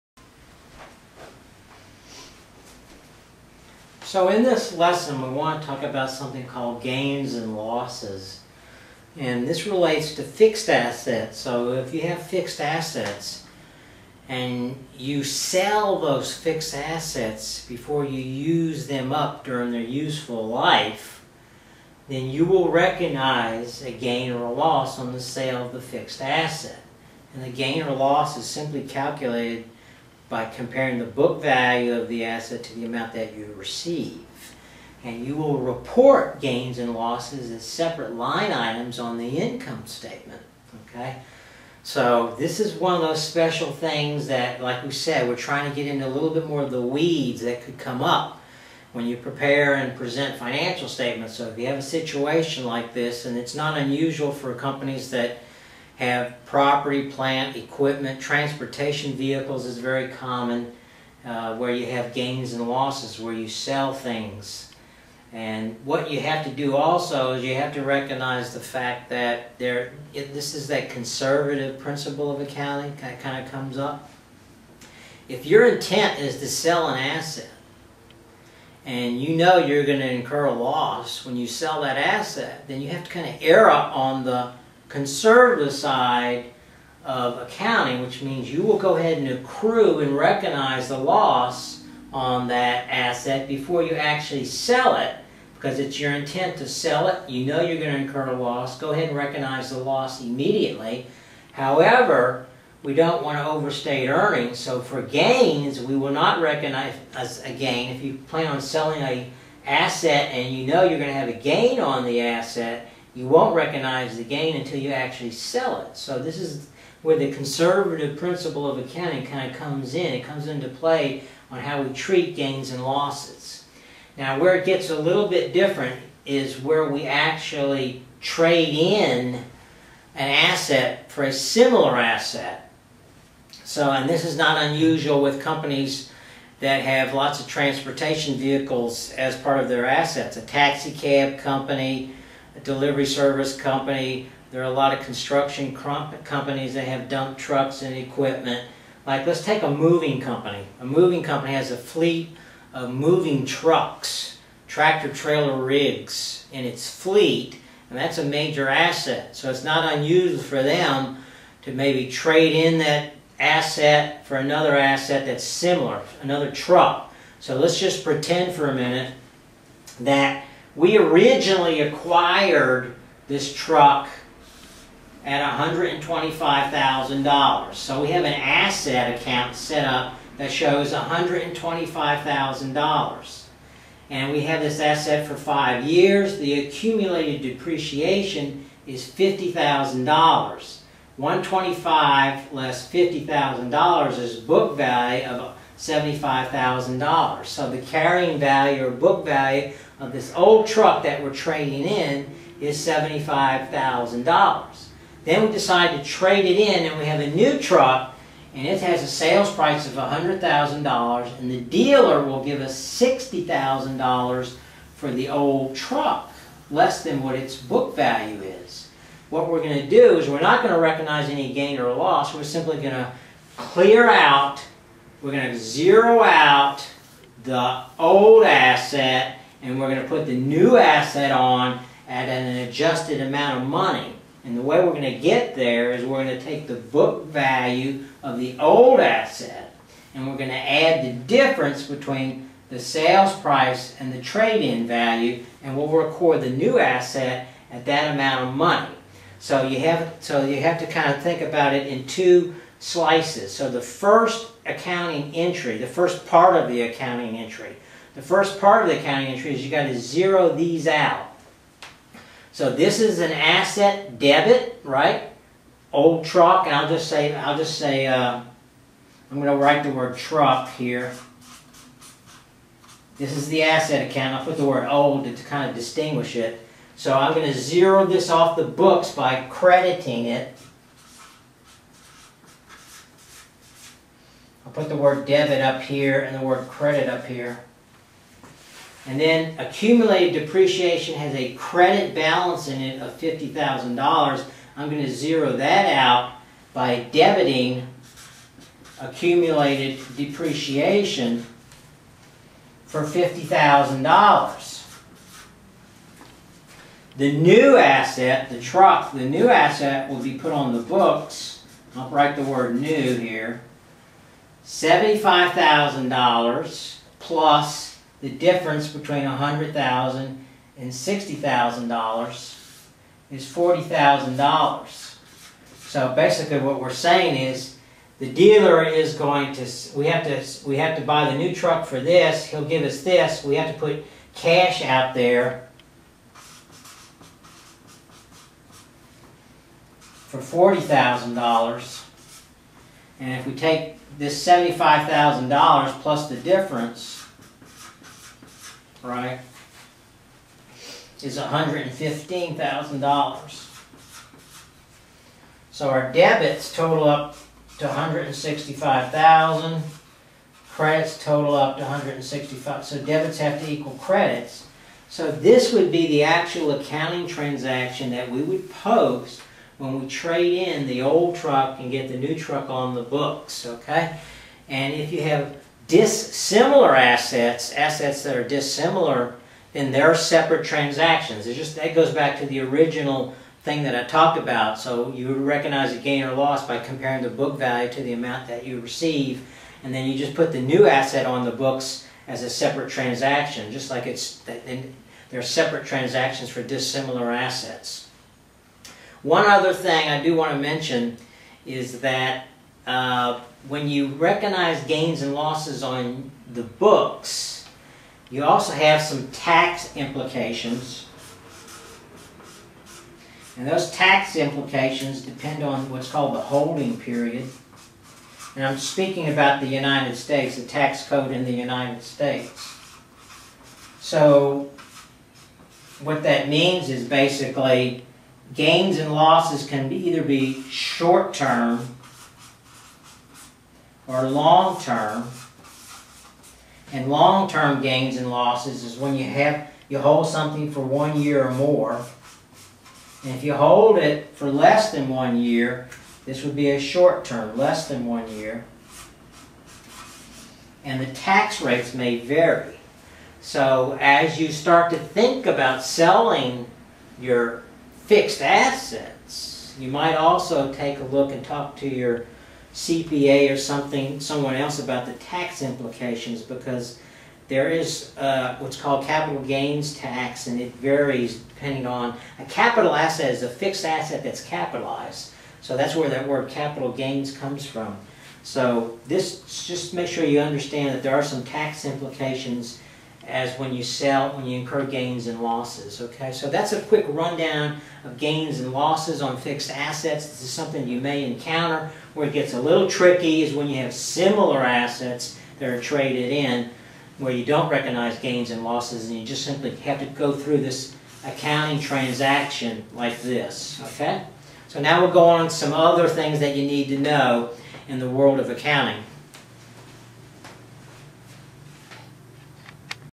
Accounting for Gains and Losses
This brief video lesson explains how to account for gains and losses upon the sale of a fixed asset. Presented by Matt H. Evans, CPA, CMA, CFM